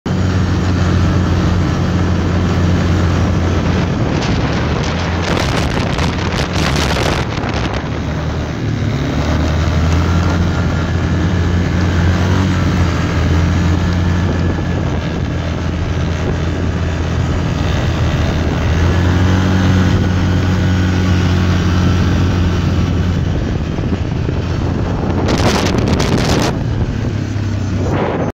Logan riding on his TTR 50
Riding at my Aunt's property in Jersey